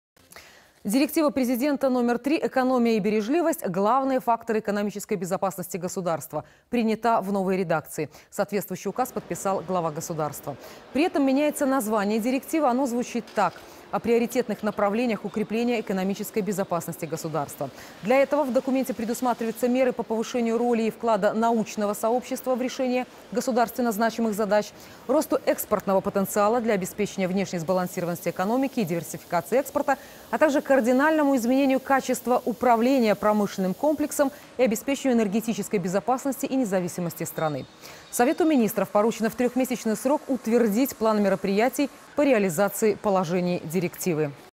Директива Президента №3 принята в новой редакции
В документе предусматриваются меры по повышению роли и вклада научного сообщества в решение государственно значимых задач